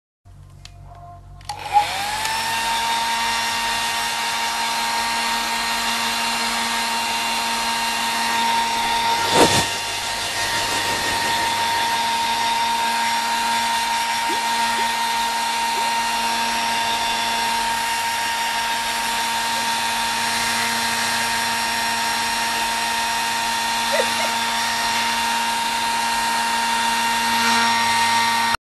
Cute Chihuahua Puppy Fighting Playing with Hair Dryer
sorry i no longer sell teacup chihuahuas. i'm moving to the states and changing my career.